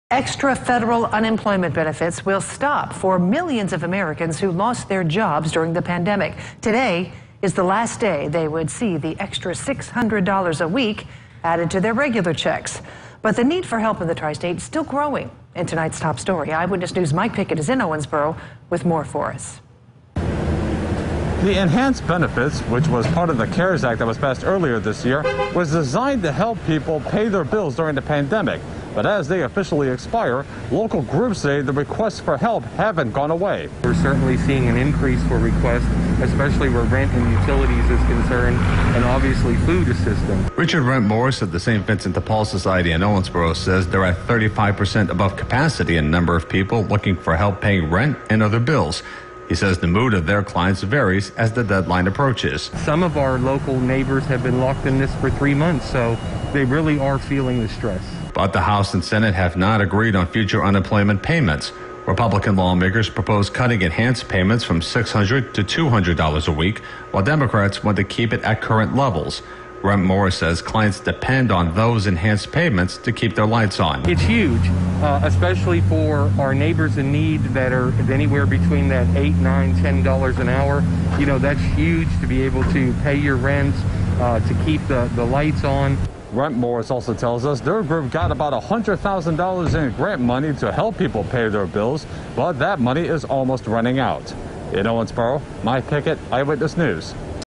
Demand for help continues as enhanced federal unemployment benefits set to expire
Extra federal unemployment benefits will stop for millions of Americans who lost their jobs during the pandemic.